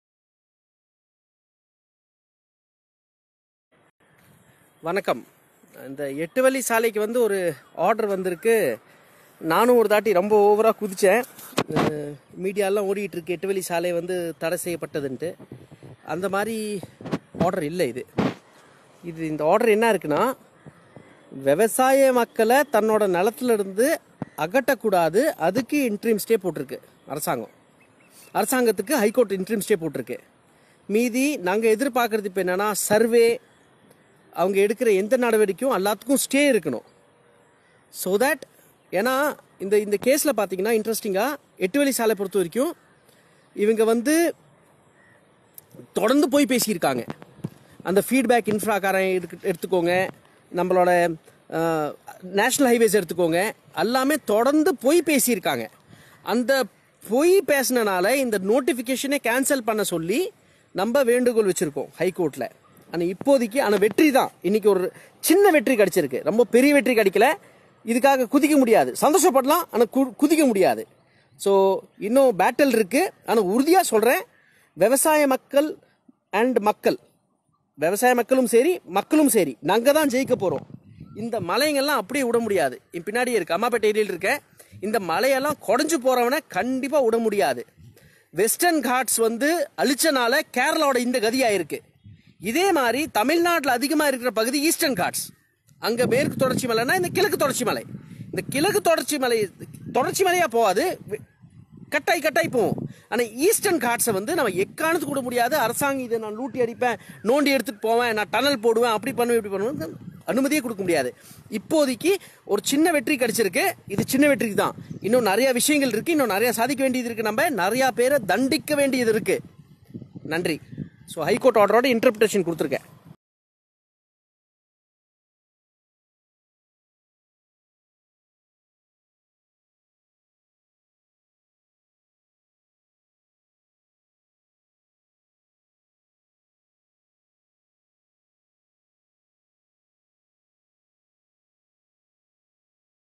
8 வழிச்சாலை விவகாரம் பியுஷ்மனுஷ் விளக்கம் | Piyush Manush on Salem 8 Way Lane | Salem 8 Way Road
Piyush Manush  is an environmental activist from Tamil Nadu, India. Along with few people in 2010, he formed the Salem Citizen’s Forum, a collective of urban citizens that engaged in people welfare activities.The Forum adopted the Mookaneri Lake, a 58-acre located at the foot of the Shevaroy Hills, was conserved from massive pollution. He has also made contributions to revive bodies of water such as Ammapettai Lake, Kundukkal Lake, Ismailkhan Lake, Arisipalayam Pond and Pallappatti well. He also opposed commercial mining in Kanjamalai hills and stood against encroachments and land mafia. He created the "Coop Forest" in 2009, an initiative for forest preservation, on 1.5 acres of hilly land in Dharmapuri. He, along with others, received the CNN-IBN Indian of the Year award on behalf people of Chennai in 2015.